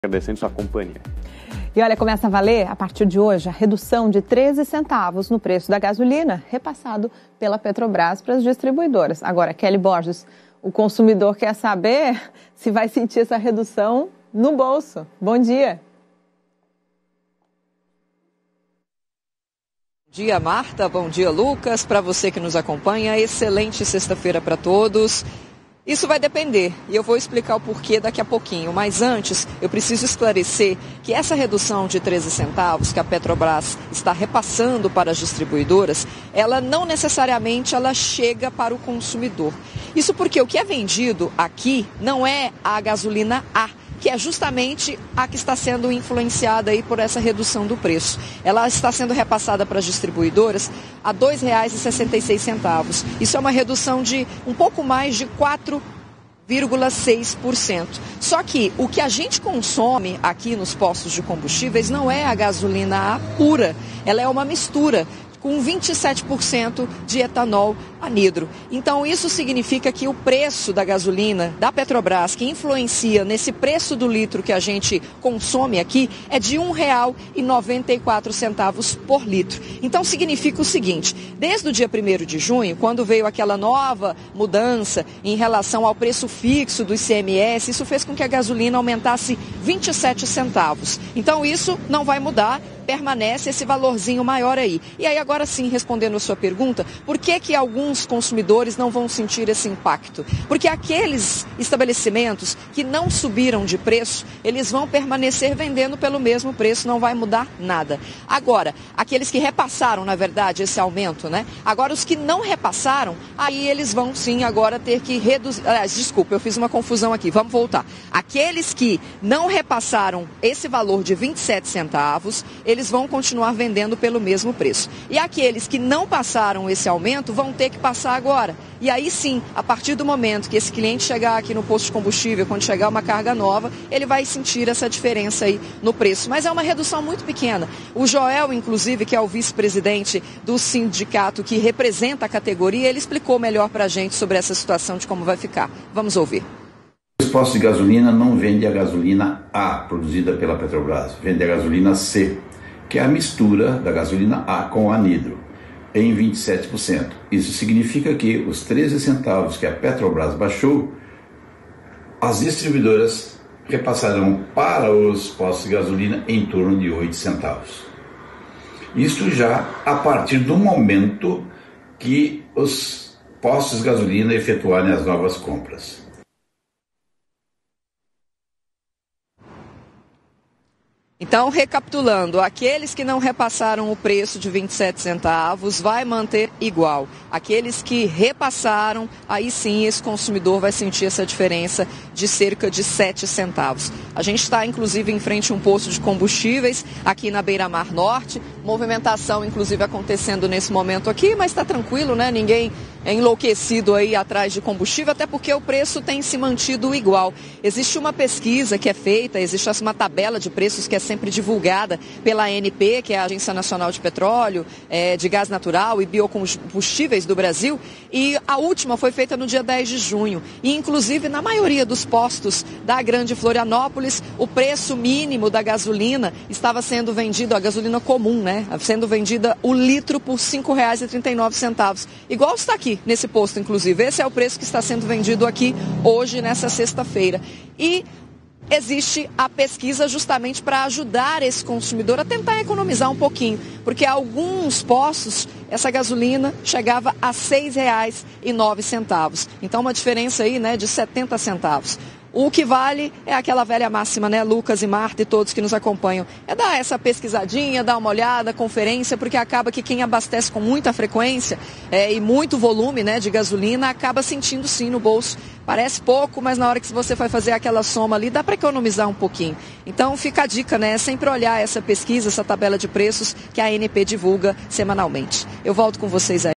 Petrobras anuncia queda no preço da gasolina a partir desta sexta-feira
A repórter Kelly Borges tem os detalhes sobre esse novo anúncio; acompanhe.